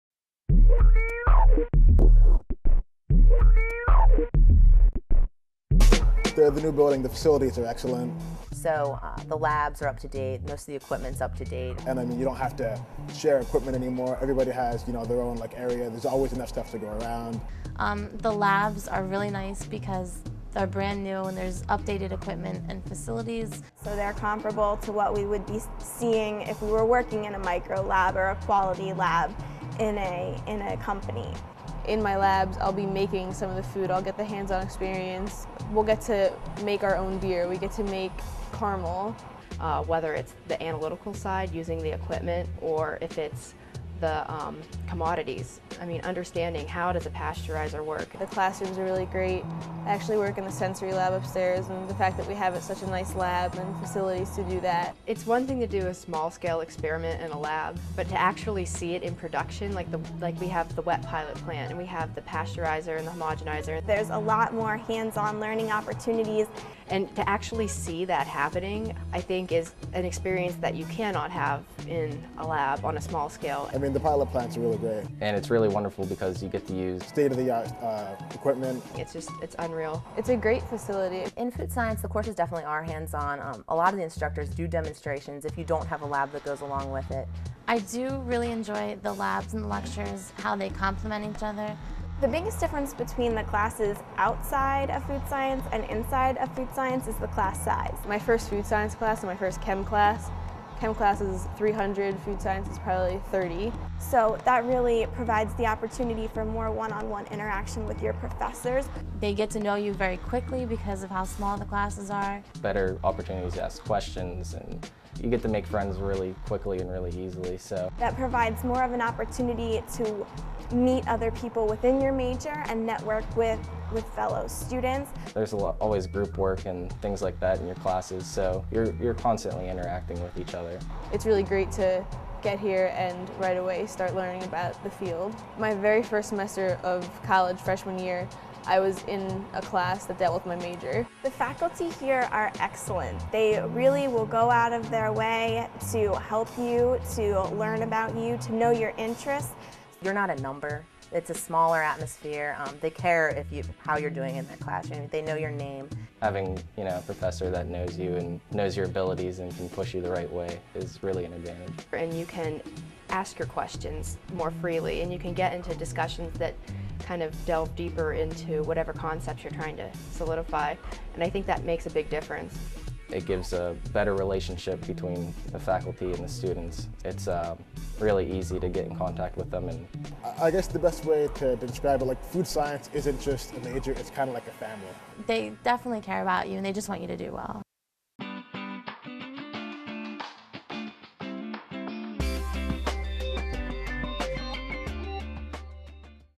Penn State Food Science and Berkey Creamery Ice Cream
See the opportunites our Food Science majors have at Penn State.  Learn about our state of the art building with cutting edge research laboratories where our undergraduate students are applying 'hands on' science to real world problems.  Like ice cream?  Who doesn't!  See the new Berkey Creamery - home of the world's greatest ice cream -  on Penn State's beautiful main campus.